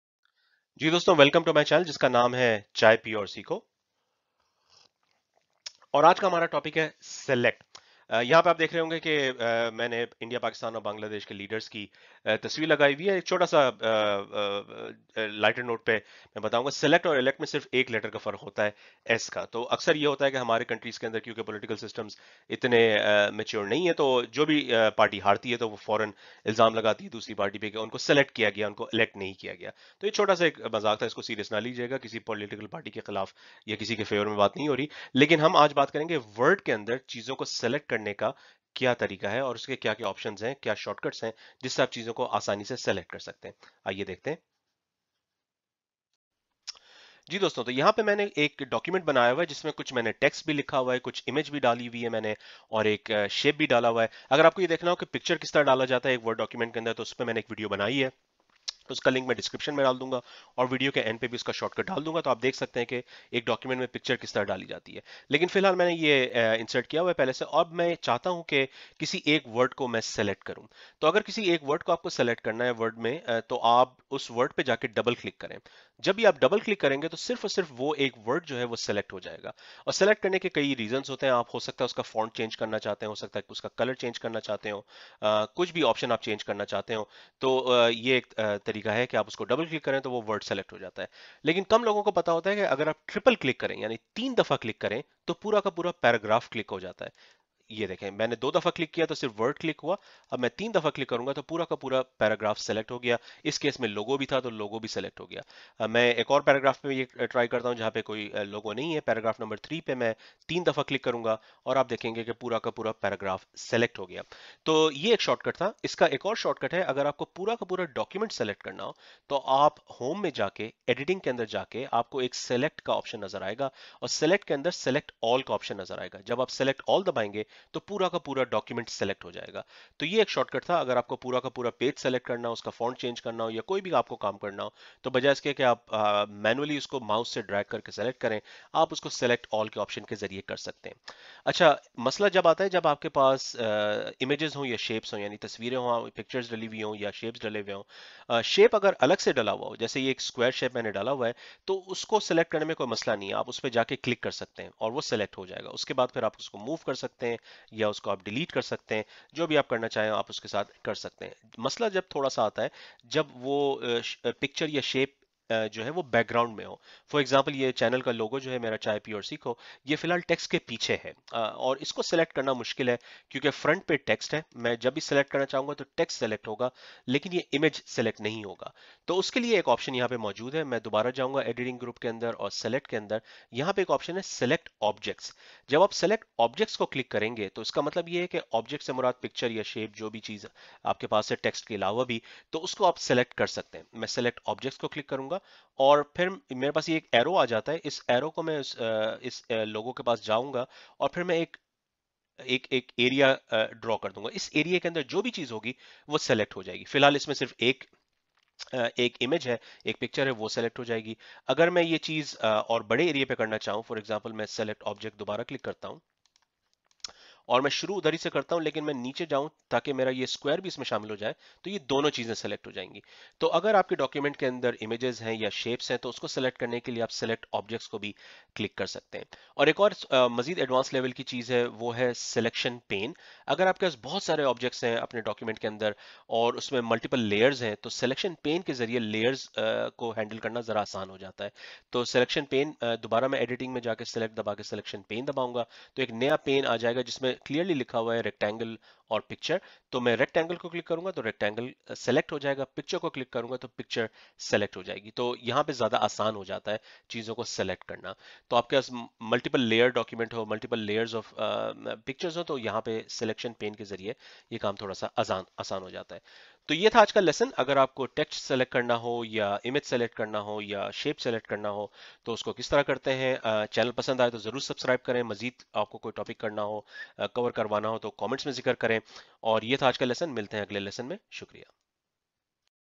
Word Level 1 - Select [ Urdu / Hindi ]
Word me paragraph aur pictrure select kerne ka shortcut